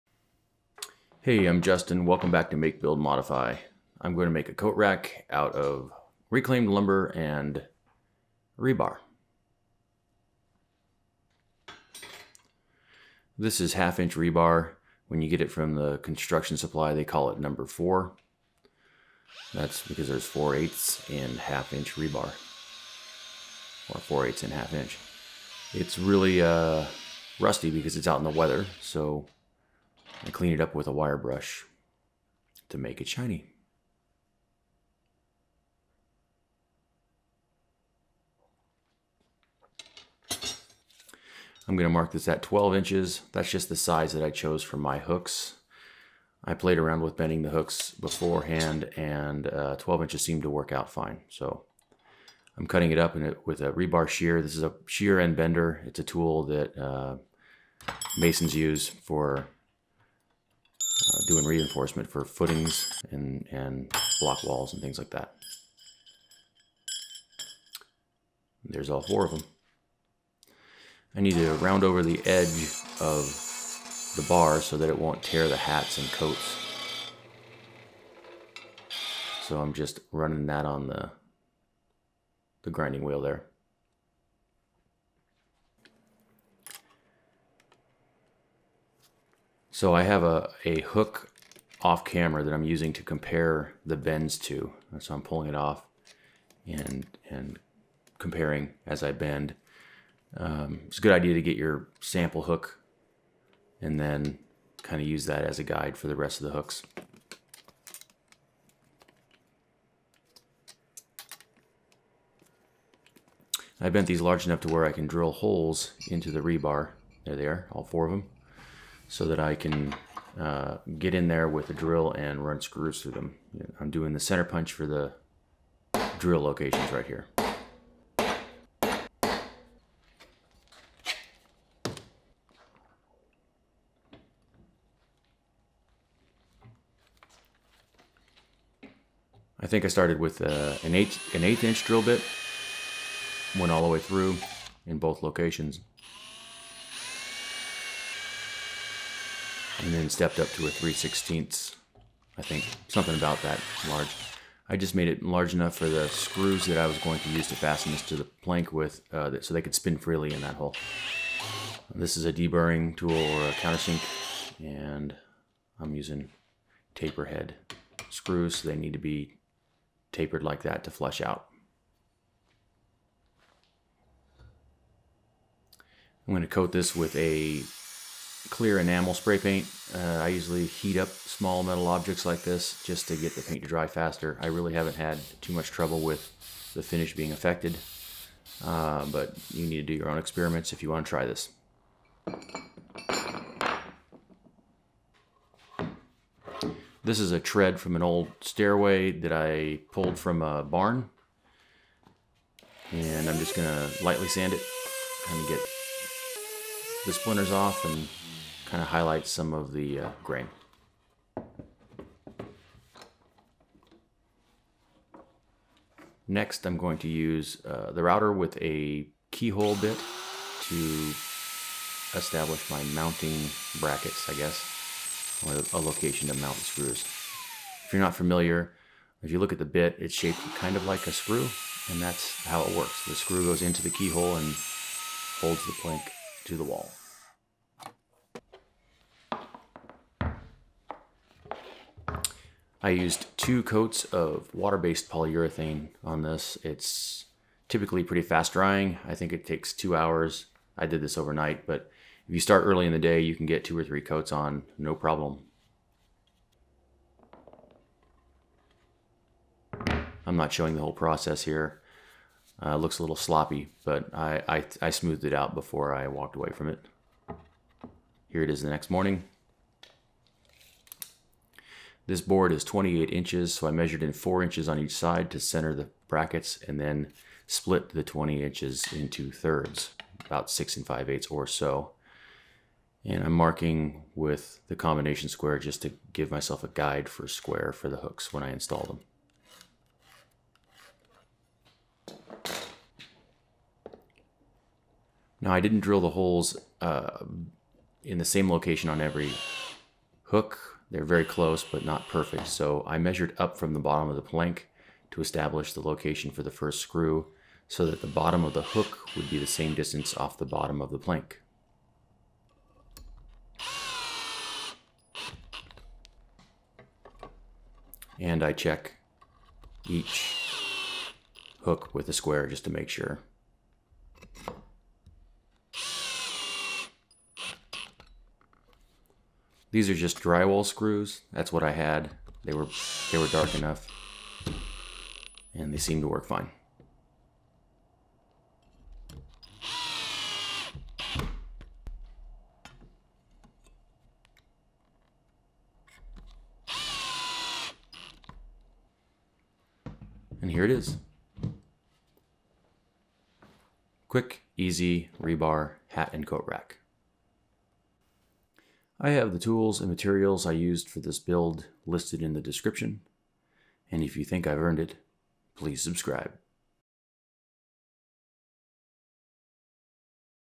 Coat & Hat Rack Made with Rebar & Barn Wood
I make a simple rebar and barn wood coat and hat rack. I use ½” rebar and a tread from an old barn staircase. This is a quick and easy build great for anyone that wants to start making stuff.

FILING, SANDING & GRINDING:
- 10" Disc Sander
- Milwaukee Cordless 18v Grinder
- 3 Inch Knotted Wire Cup Brush
- Milwaukee Palm Sander

HAMMERS AND POUNDING:
- 4 Pound Mini Sledge

DRILLS, BITS, ROUTERS, and DRIVERS:
- High-Speed Steel Drill Bits
- Milwaukee Drill combo Kit
- Countersink Drill Bit
- Center punch set
- Porter Cable Router
- Freud Key Hole Bit

PENS/PENCILS AND MARKING:
- Sharpie Marker

RULERS AND SQUARES:
- Stainless steel Ruler
- Tape measure
- Swanson Combination Square

SHEARS, SCISSORS & CUTTERS:
- Rebar cutter and bender

STORAGE AND TOOL ORGANIZATION:
- Roll-out Bedslide truck bed

CLAMPING, VISES and GRIPPING:
- Bench vise
- 6 inch bench vise

WELDING, BRAZING & SOLDERING:
- Bernzomatic Trigger Start Torch acetylene torch kit

FINISHES, PAINT, COATINGS, AND PAINTING TOOLS
- Minwax Polycrylic Satin
- Krylon Crystal Clear